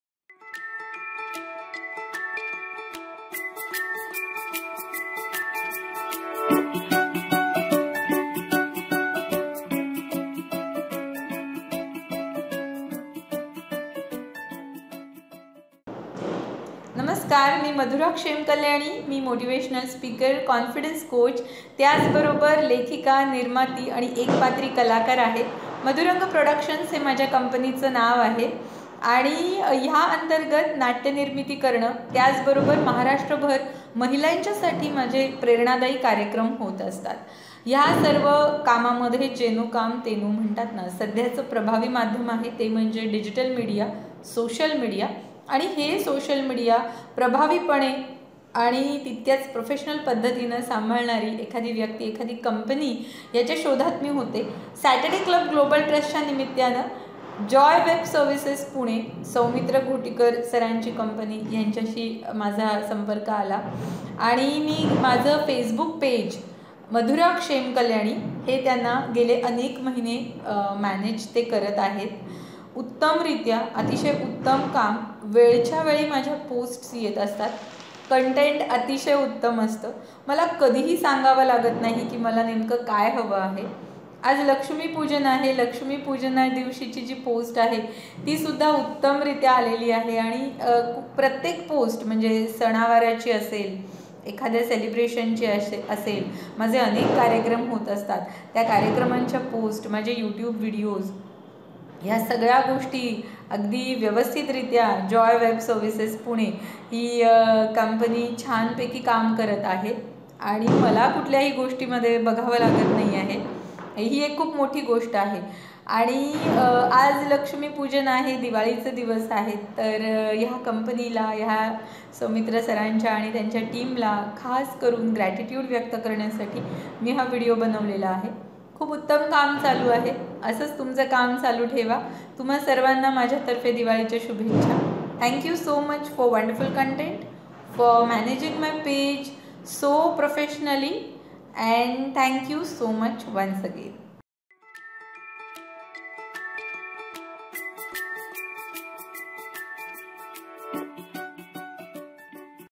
Digital Marketing Services Review by Madhura Kshemkalyani
Ms Madhura Kshemkalyani is a Professional Business Coach from Nashik. She recently reviewed our Services. We thank her very much indeed